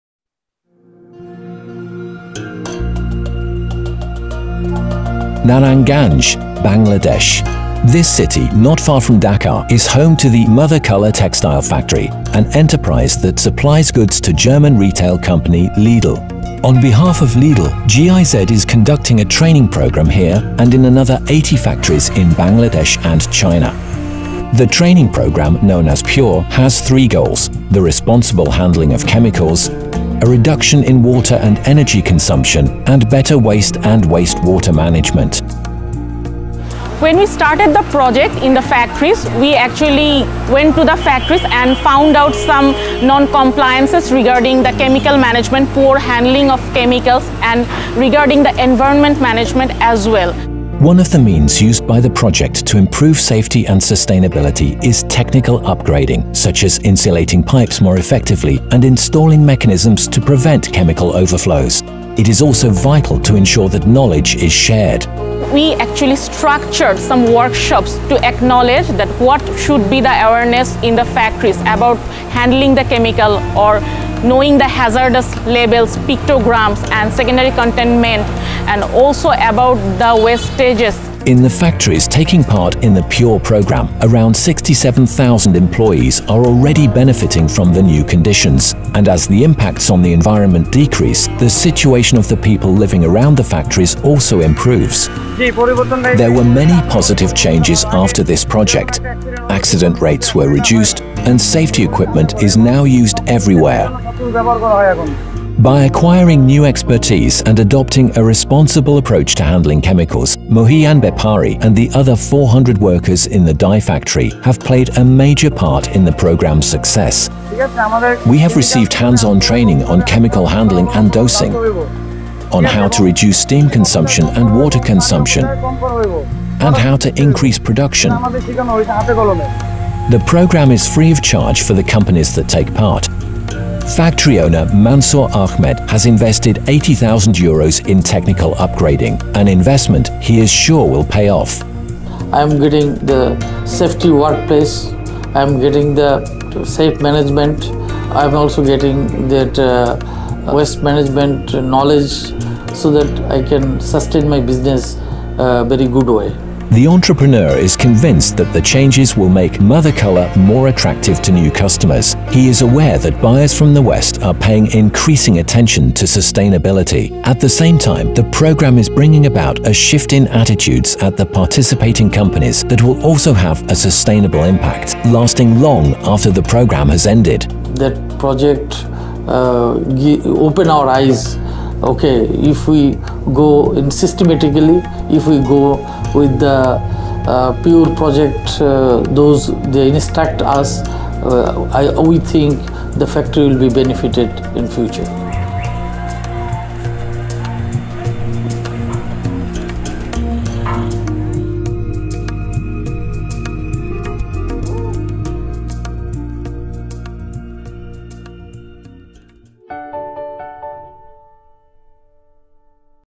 Le programme de formation PURE
PURE (Projekt für Umwelt und Resourcenefficienz, projet pour l'environnement et une utilisation plus efficace des ressources) est un projet mené dans 80 usines textiles en Chine et au Bangladesh. Ce programme de formation gratuit a pour but d'améliorer la sécurité et la durabilité dans ces usines. Dans le cadre de formations sur le terrain, les ouvriers apprennent à manipuler les produits chimiques en toute sécurité et à réduire la consommation d'eau par différentes interventions.